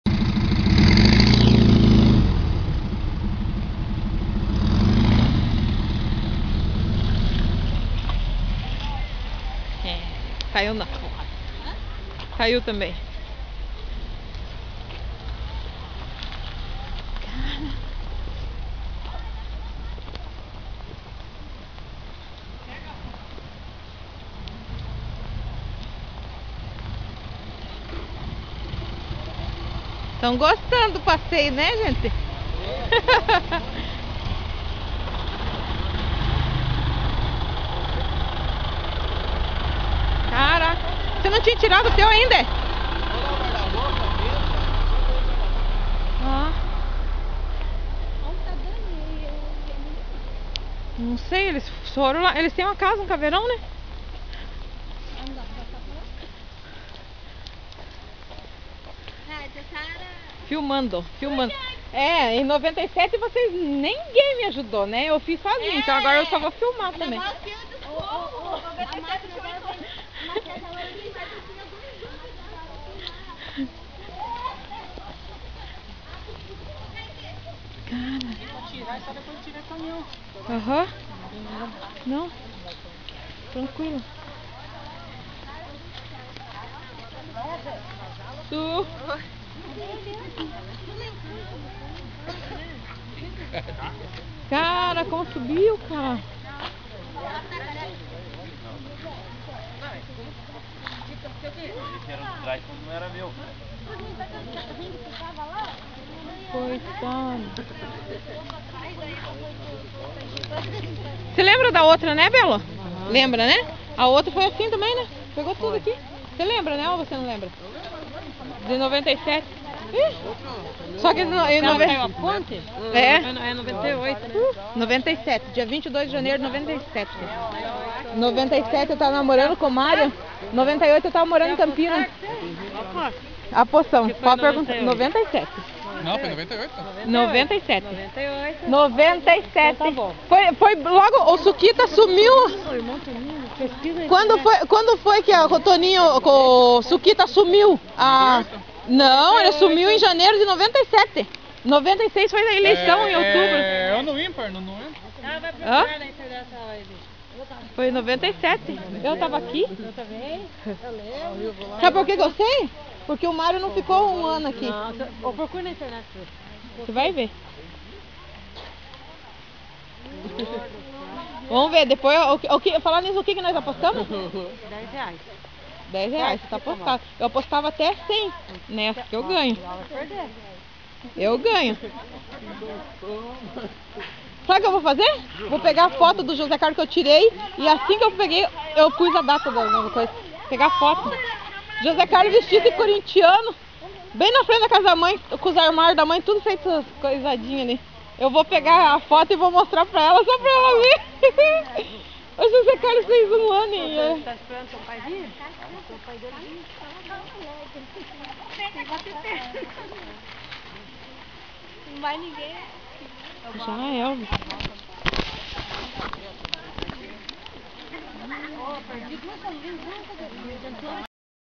ENCHENTE EM ADRIANÓPOLIS 2011
ESSA FOI MAIOR DO QUE A DE 1997(22/01/1997) NÃO QUEBROU TABOO SÓ CONFIRMOU APÓS 14 ANOS MAIS UMA ENCHENTE! (01/08/2011)